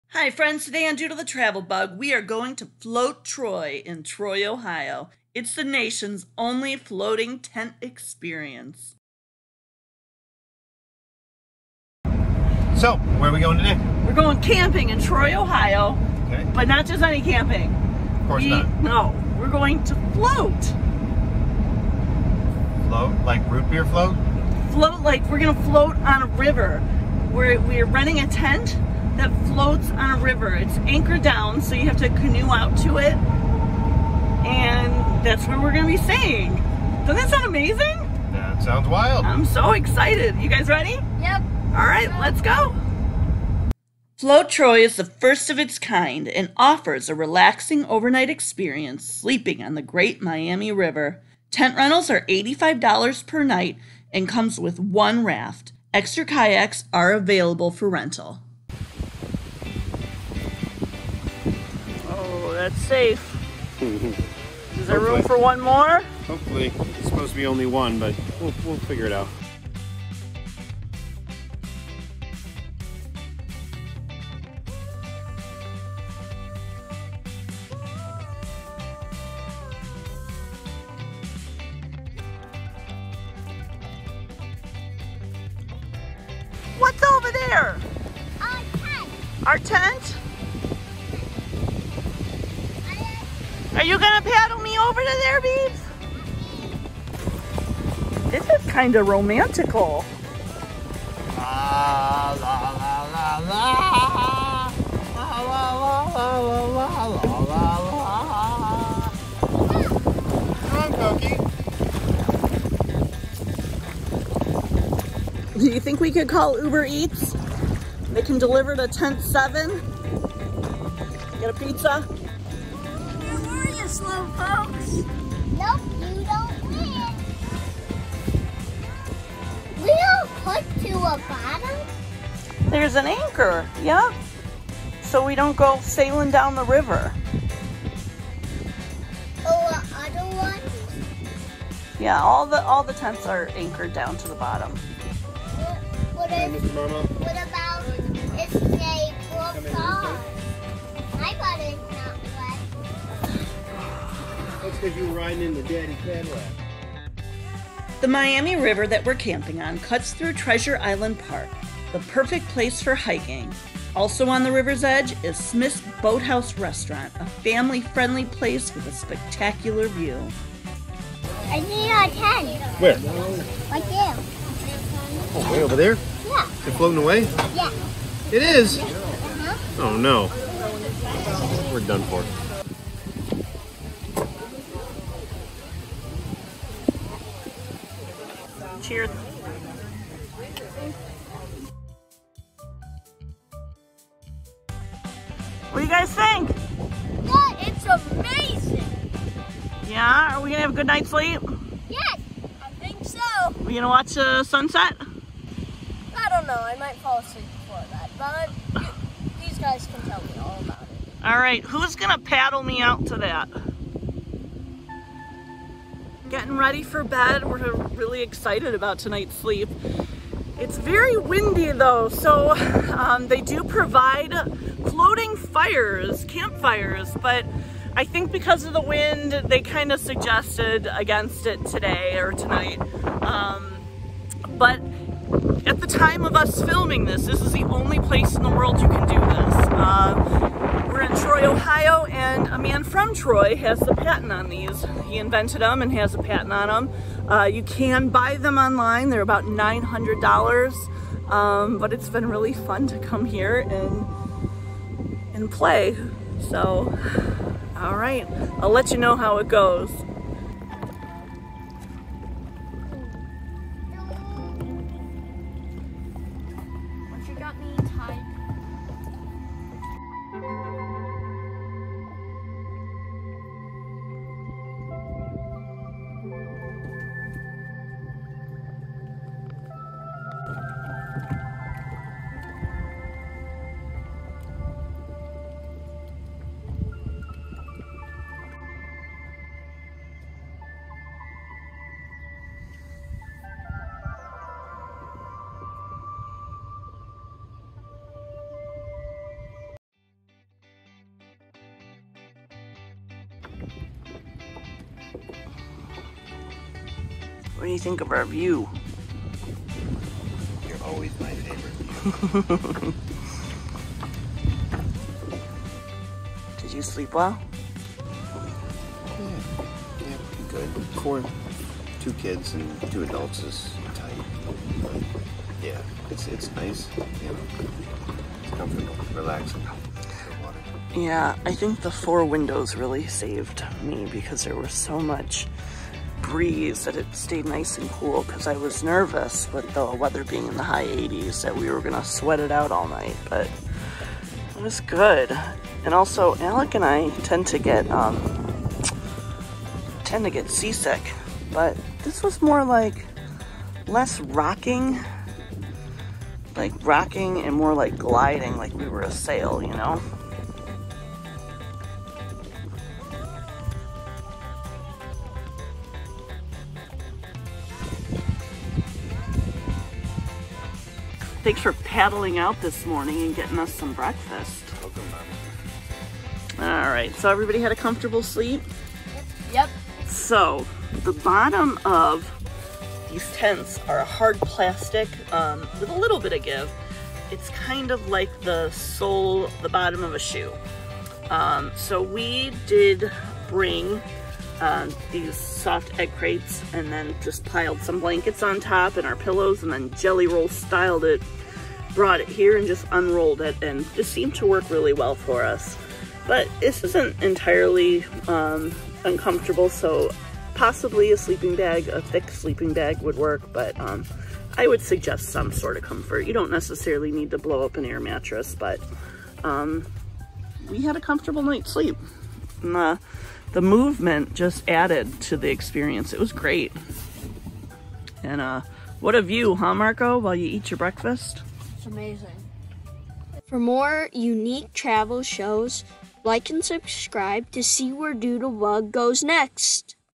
FLOATING TENT! Unique Camping at Float Troy, Ohio - Camping on the Water.
We FLOAT on the Great Miami River in Troy, Ohio for the most unique camping experience anywhere! Camping on the water!!!   Float's floating tents are the first of it's kind anywhere in the world.  The tents are anchored in the Miami River that cuts through Treasure Island Park in Troy.  Water Camping or River camping is the most relaxing way to camp.
    These are Shoal floating tents that are inflatable floating rafts with tent toppers that allow campers the most unique camping experience of camping on the water.  The tents are manufactured by Smithfly right in Troy.
     For $85 a night, campers are provided with a floating tent that sleeps up to 4 adventure campers, a raft, floating fire pit and life jackets.  Extra kayaks are available for rental.  Campers must row themselves and their gear out to their floating tent.  Bathrooms are located on land.  We brought our old toddler potty  with us for emergency bathroom situations.
     We enjoyed the surrounding park and Smith's Boathouse Restaurant.  Smith's Boathouse Restaurant is located in Treasure Island Park right on the water.  It is a family friendly place that has spectacular views of the river and our home for the night, our one of a kind floating tent.
     Our River Tents swayed us to sleep as the river breeze came in through the four windows.  This was an incredibly different and unique camping experience that we will never forget!

Float Troy